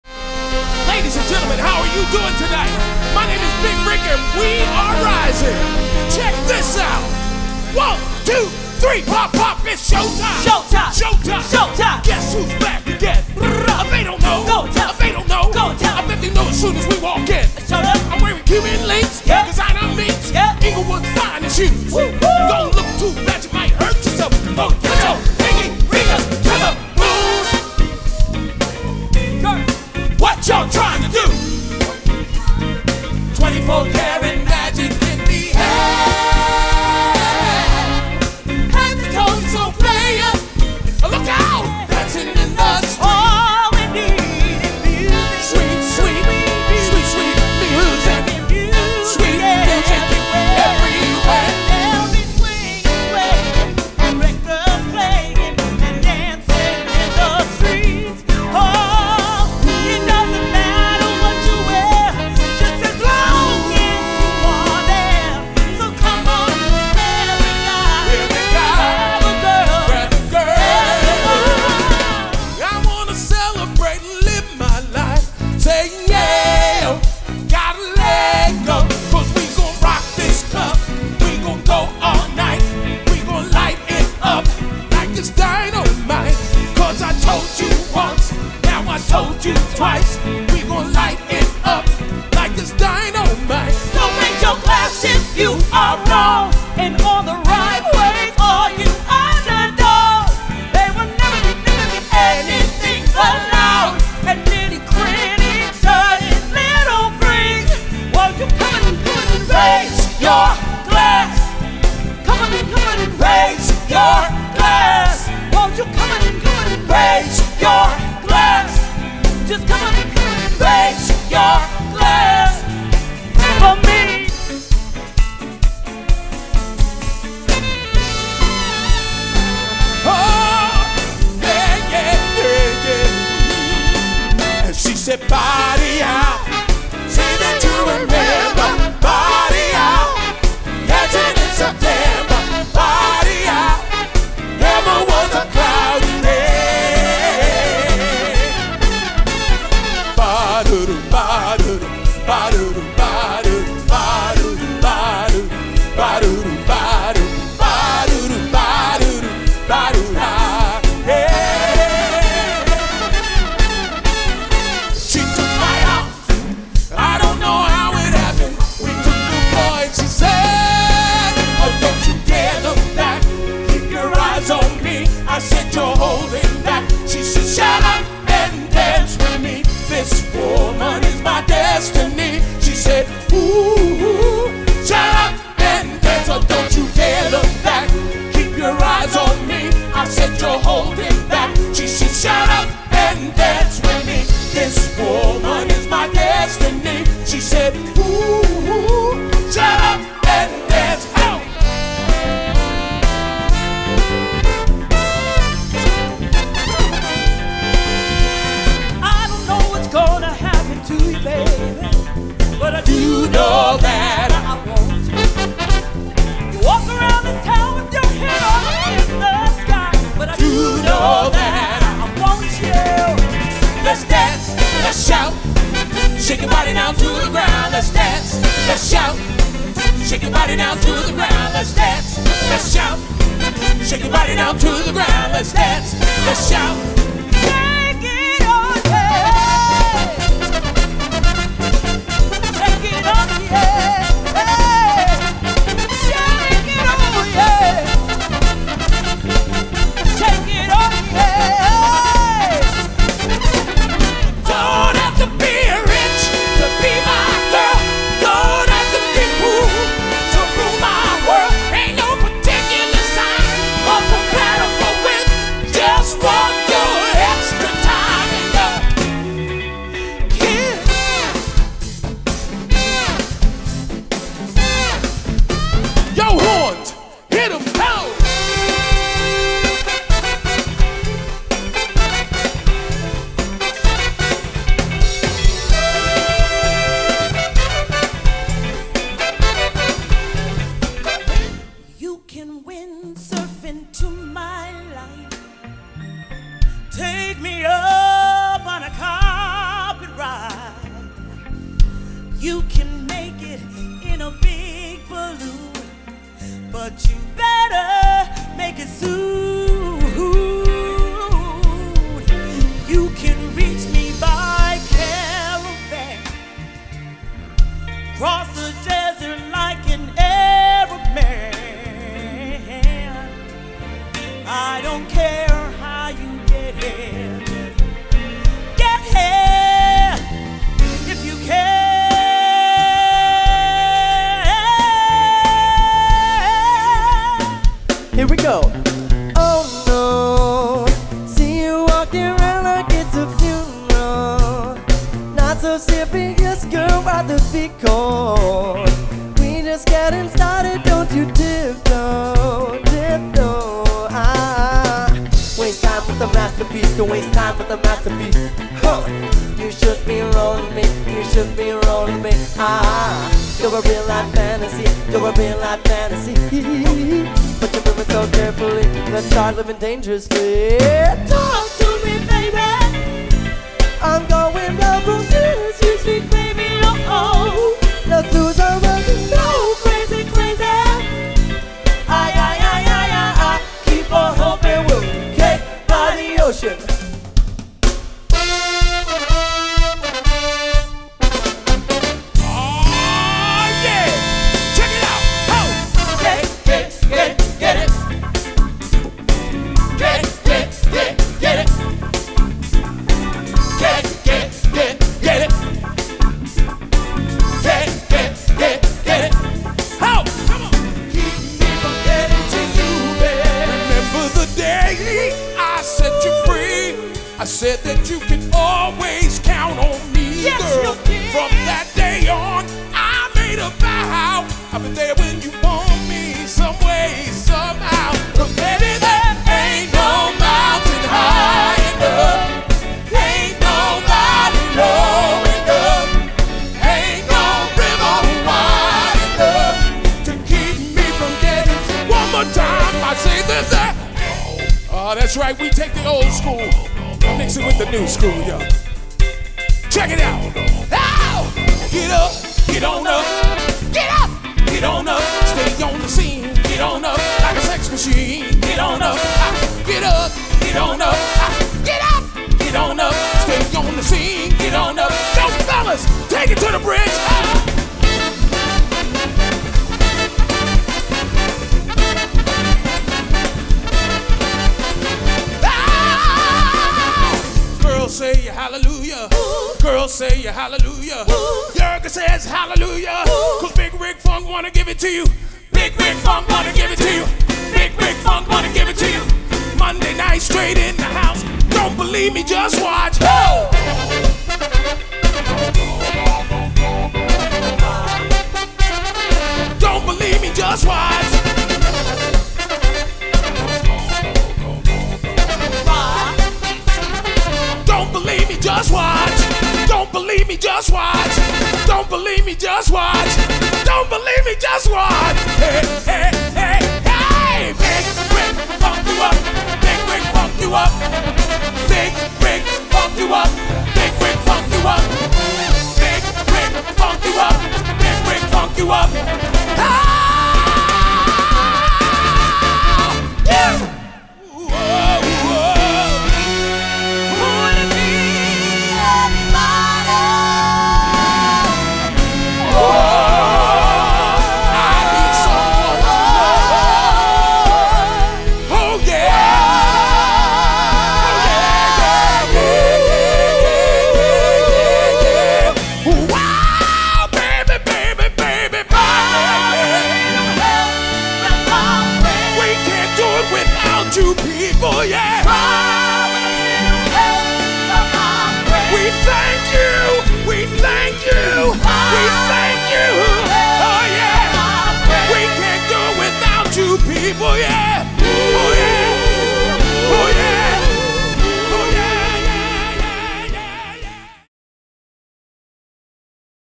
BVTLive! Big Ric Rising - Philadelphia Event & Wedding Band Full Promo Video
If you want stage presence and interaction at your wedding or event you will certainly find it with Big Ric Rising. Not only do they provide a spectacular show with their world class vocals and horns section, but they also bring the performance full circle with their unmatched energy and interactive show. Big Ric Rising makes you want to get up on your feet and dance along to their music, as every good party band should! And that is what makes a party memorable, right?! Their song list includes several genres from old to new, for guests of all ages to enjoy. If you want a dynamic high-energy band, Big Ric Rising is perfect for you!

For more information about BVTLive!